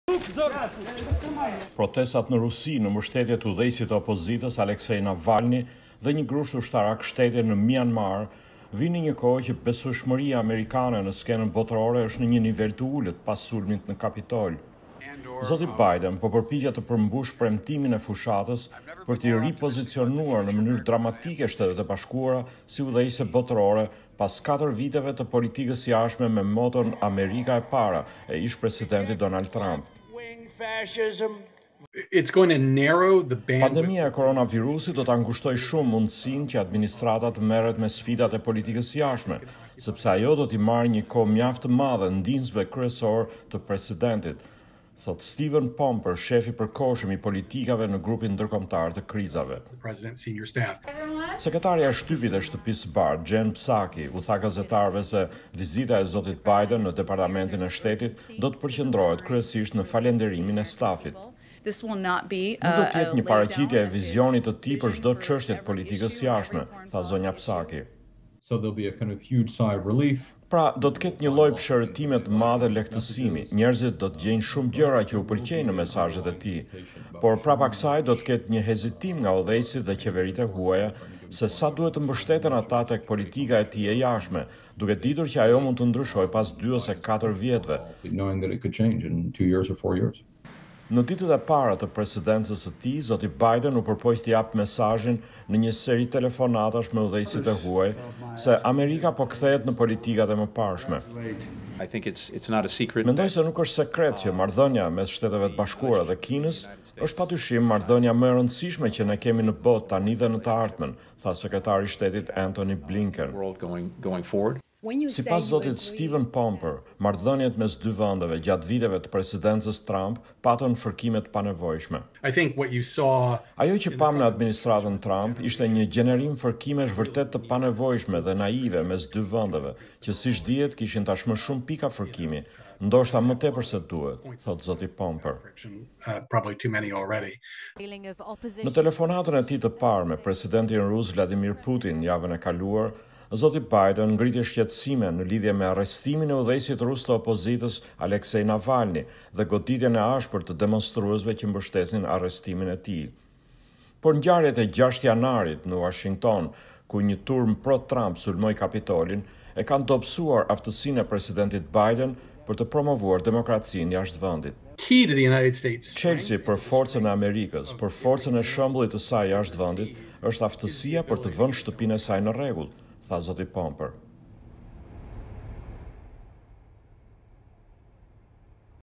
Biden dhe politika e jashtme
Presidenti Joe Biden pritet të parashtrojë pikat kryesore të politikës së jashtme të administratës në një fjalim që do të mbajë të enjten përpara diplomatëve amerikanë në Departamentin e Shtetit. Fjalimi vjen ndërkohë që ai po përballet me dy prova në politikën e jashtme dhe kur trazirat e 6 janarit në Kapitol mund ta kenë dëmtuar imazhin e Amerikës si udhërrëfyes për demokracinë.